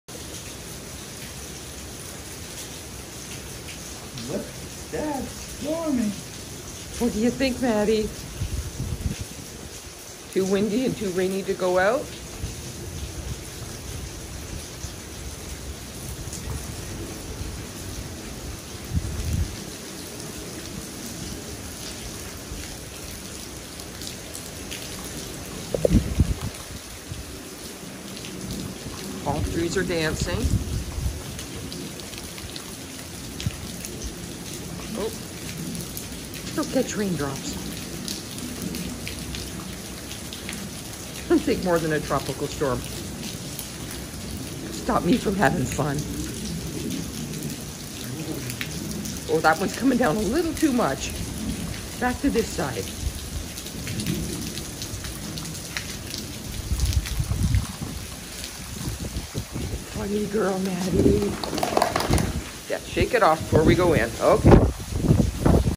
Water Loving Great Dane Catches Raindrops During Tropical Storm Nicole In Florida
Maddie the Great Dane loves to catch raindrops.  Thankfully Tropical Storm Nicole continues to downgrade as it moves across Florida.  The rain we received was similar to our summer rain storms, so it was the perfect rain for Maddie to catch raindrops.